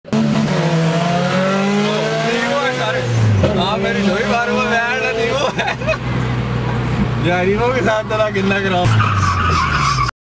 Asfand's D15 Civic vs Hamza's Evo
Just spotted Hamza's Evo and Asfand Floored it on my saying so :)
Don't mind Asfand going all nutts when he realises it's Hamza's Evo not something he can chewup and swollow :D